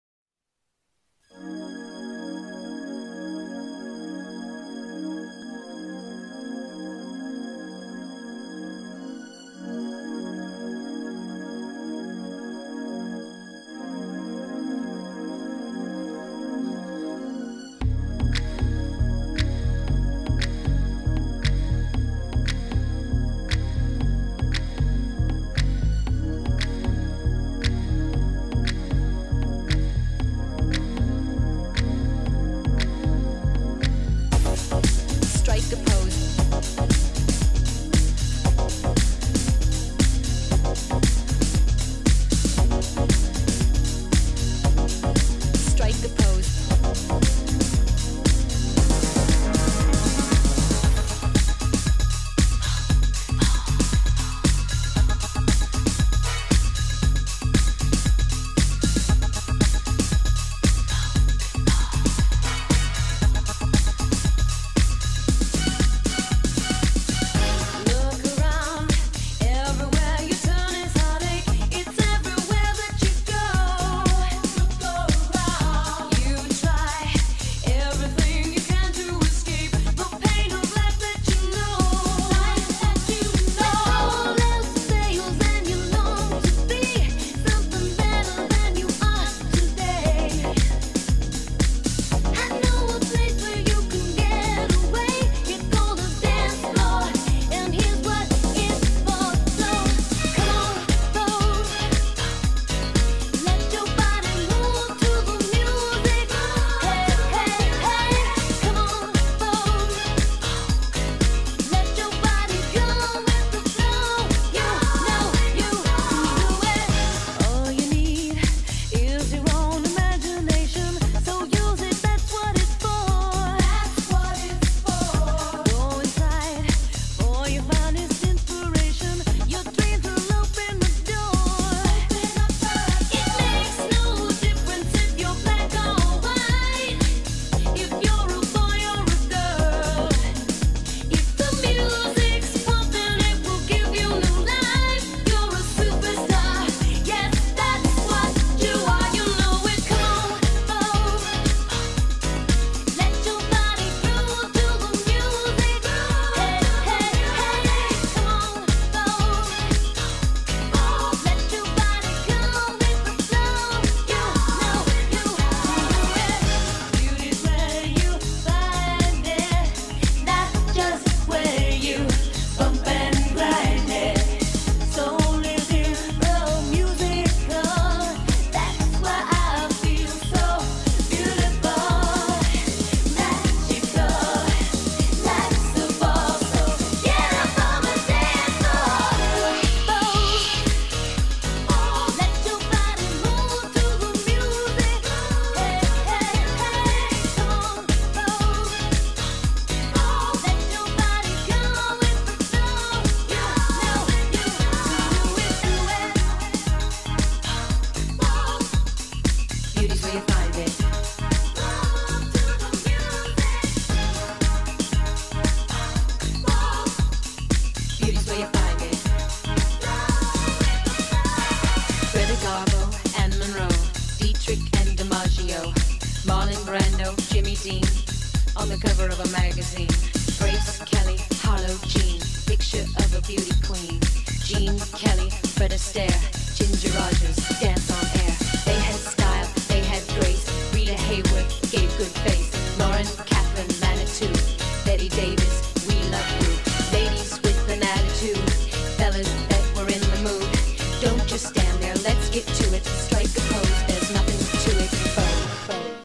გვანცა ციხელაშვილი
Georgian dancer Gvanca Tsikhelashvili did choreography of Vogue.